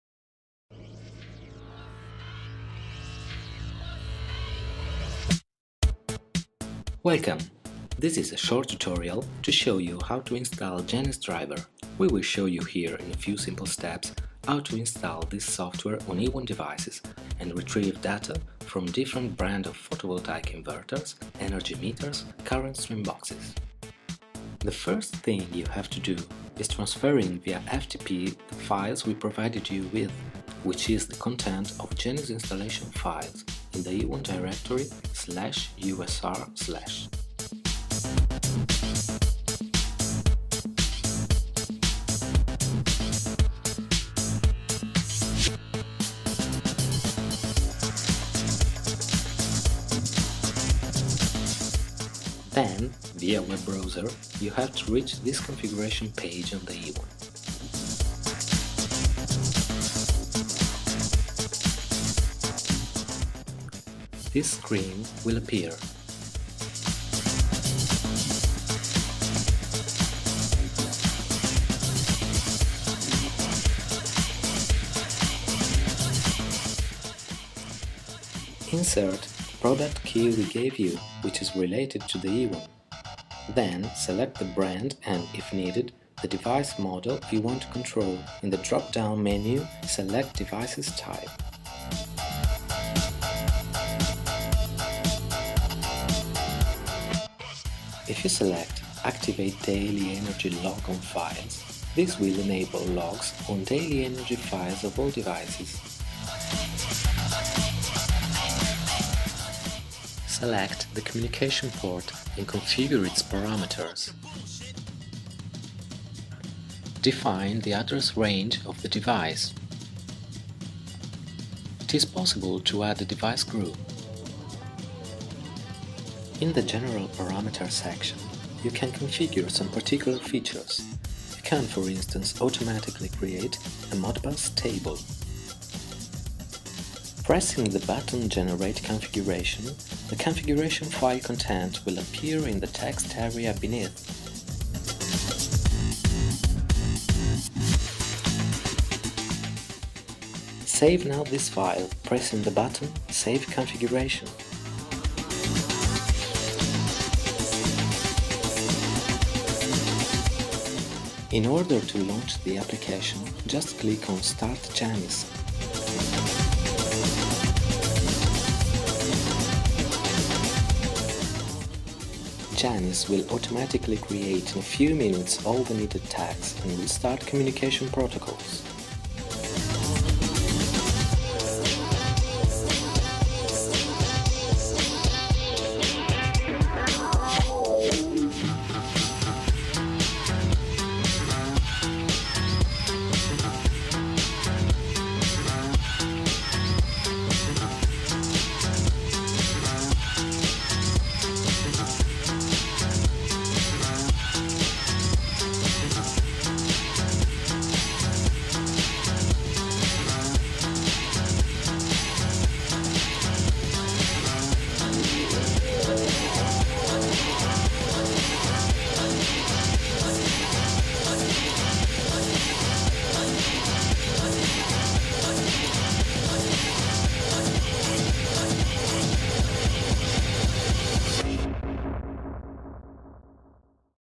Janis Software Overview
This is a short tutorial to show you how to install Janis driver, a software developed by R.S. We will show you here in a few simple steps how to install this software on eWON devices (provided with the java virtual machine) and retrieve data from different brand of PV inverters, energy meters, current string boxes.